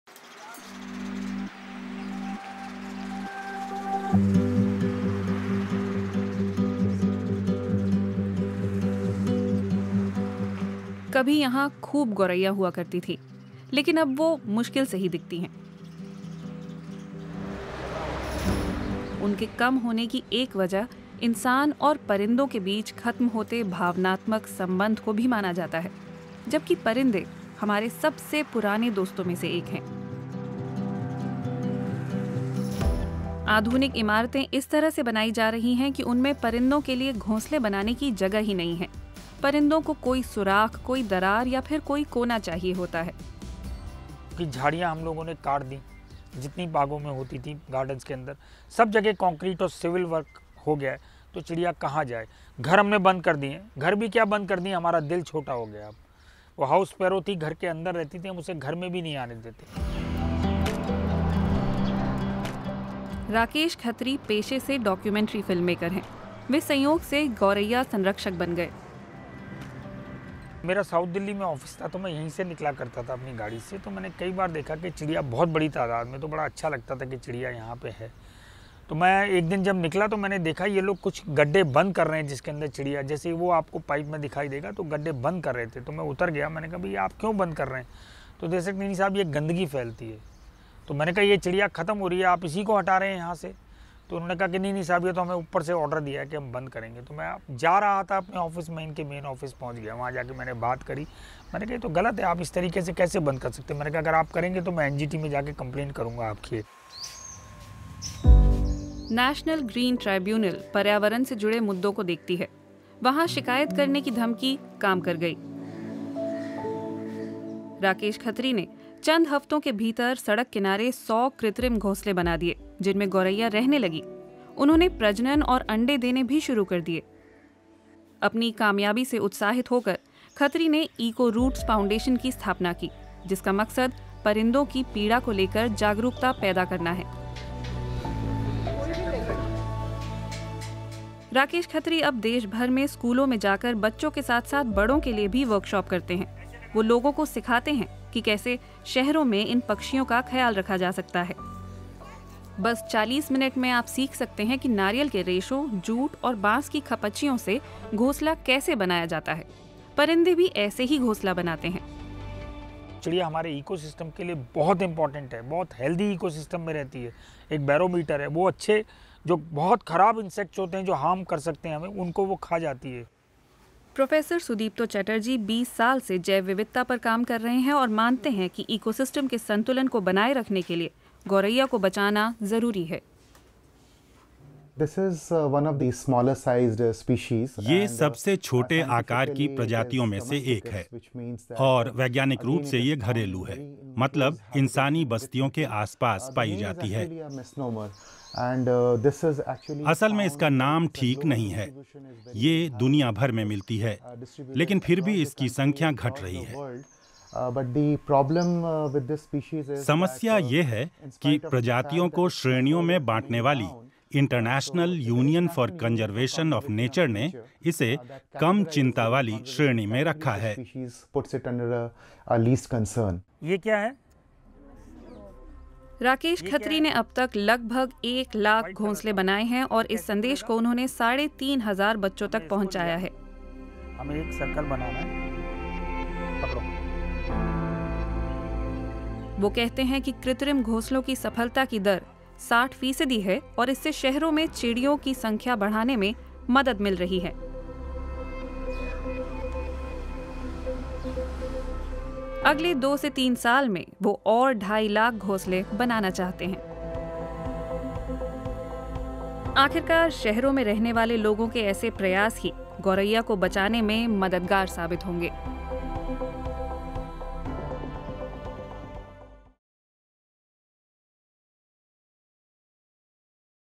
कहां गायब हो गई गोरैया [Birds help preserve the planet]
शहरों में कंक्रीट के जंगलों ने आज काफी हद तक असली जंगलों को लील लिया है. जाहिर है कि पेड़ पौधों पर निर्भर करने वाले जीवों को इससे खासी परेशानी हुई है. गौरैया जैसी चिड़िया दिखना जहां आम हुआ करता था, वे आज ढूंढने से भी नहीं दिखतीं. जब उनके घोंसले बनाने के लिए कोई जगह नहीं बची तो दिल्ली के एक शख्स ने इसके लिए कुछ करने की ठानी. नारियल के रेशों, जूट और बांस से उन्होंने खुद गौरैयों के लिए घरौंदे बनाना शुरु कर दिया. देखिए कैसा हुआ है उनकी कोशिशों का असर.
ऐसे ही और दिलचस्प वीडियो देखने के लिए यहां भी आएं..